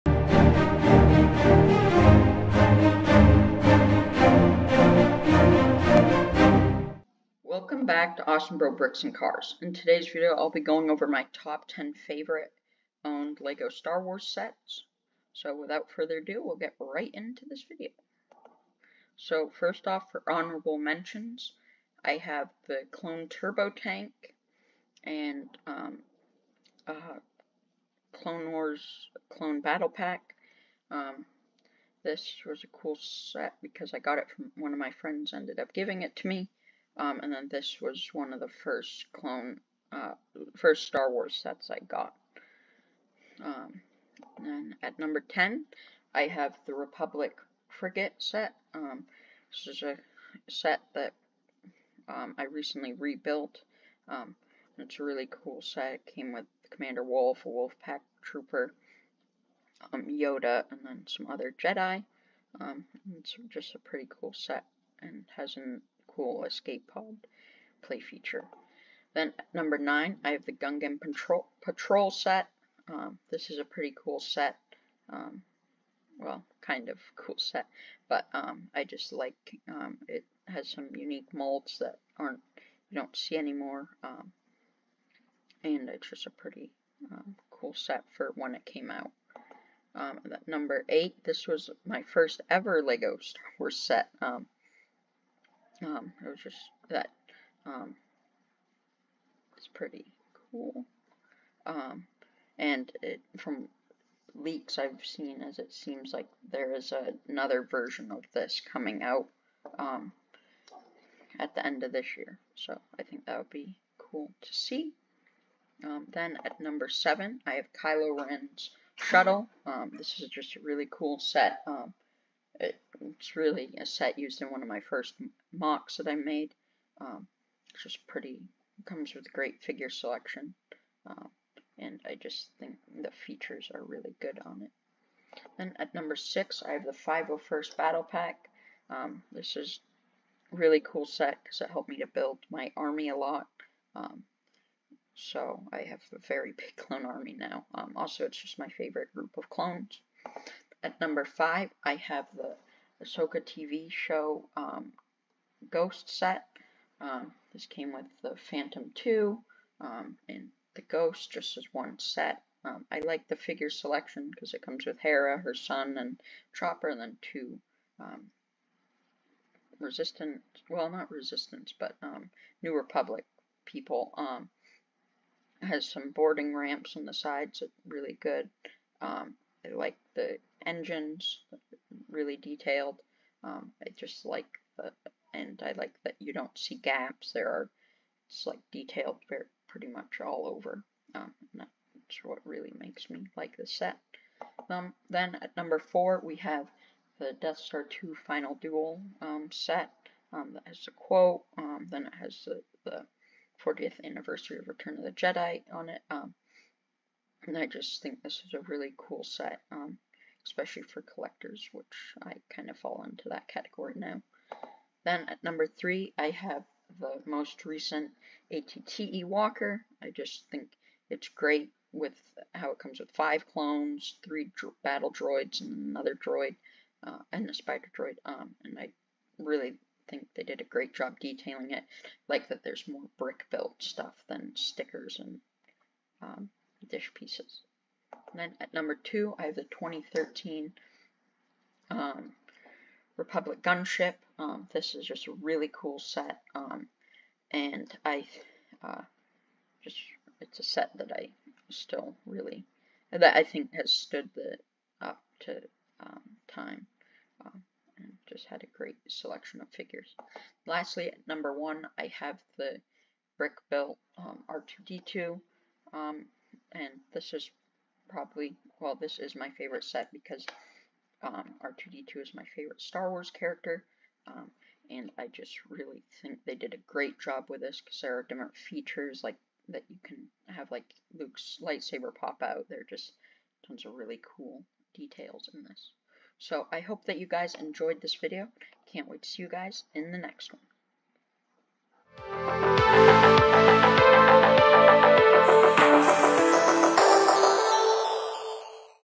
Top 10 Favorite Owned LEGO Star Wars Sets
This is the video where I go over my favorite Owned LEGO Star Wars UCS sets. Make sure to put video and MOC ideas that you think I should do next.